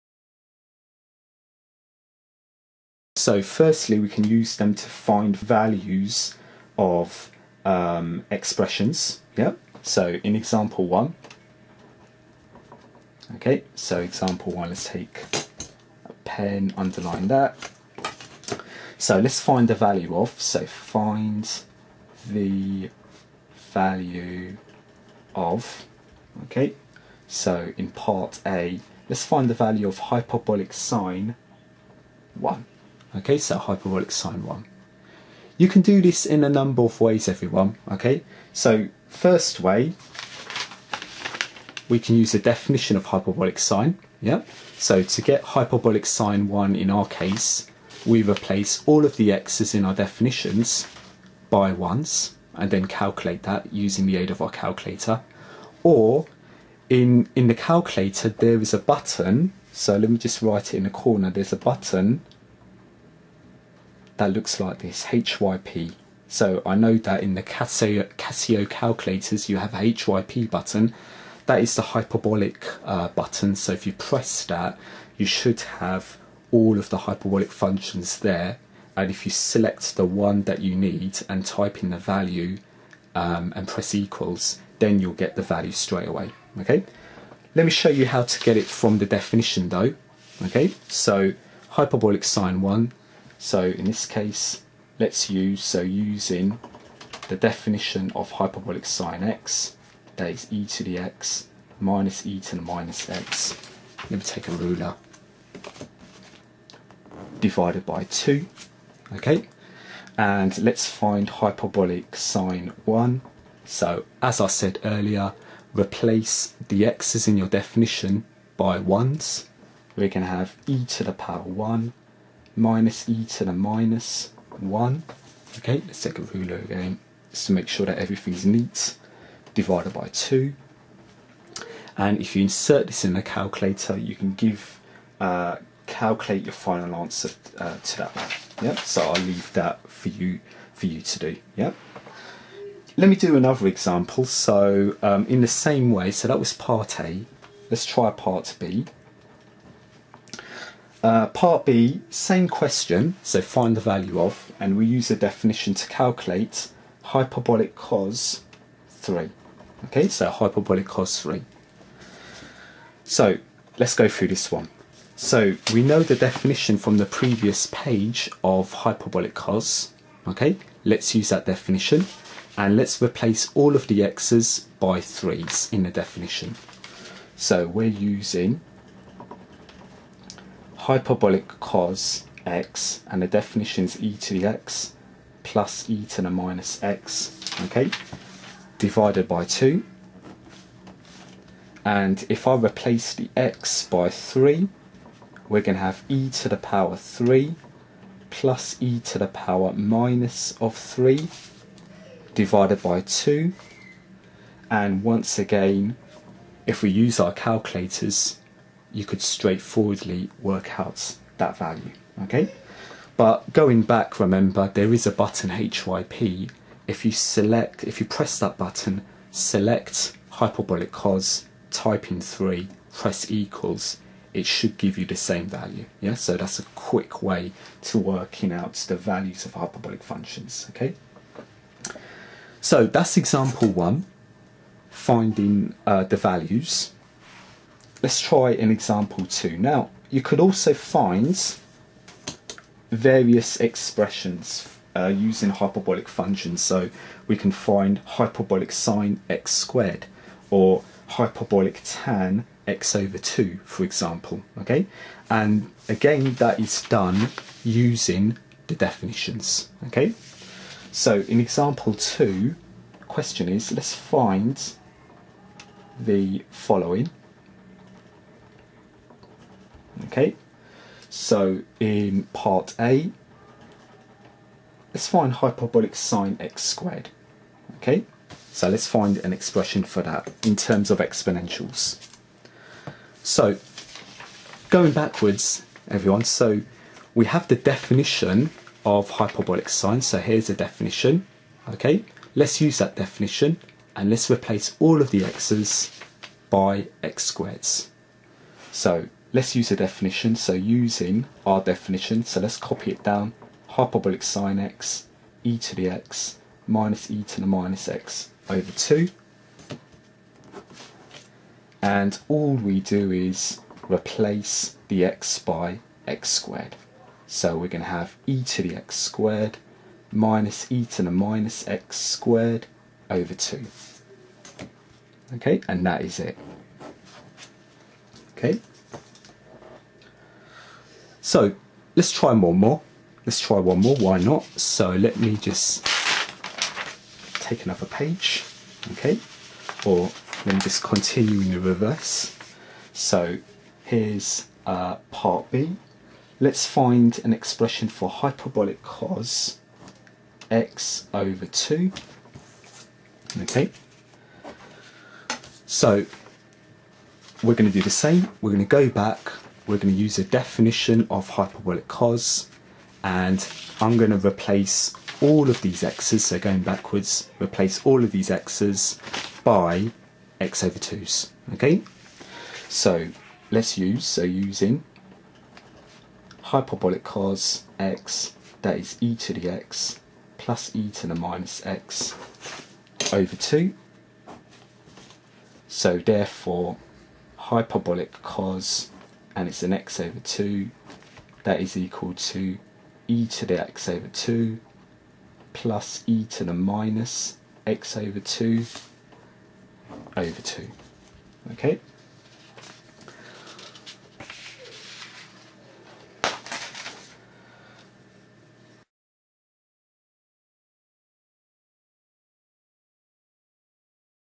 AS/A-Level Maths Hyperbolic functions - Simple examples
This AS/A-Level Maths video tutorial continues from the previous uploaded video on the definitions of Hyperbolic functions